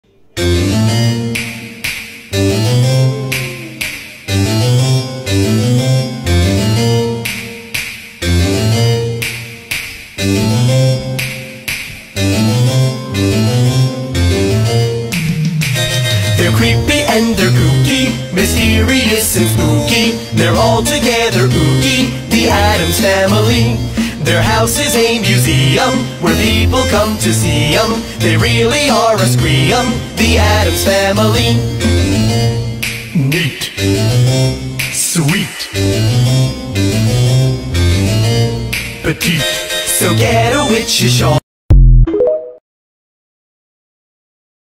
Wednesday,thing & her cello
it was going to be a rainy day the next day and we didn't want to pick a soaking tree so we went out for a night time shopping trip to terra garden center.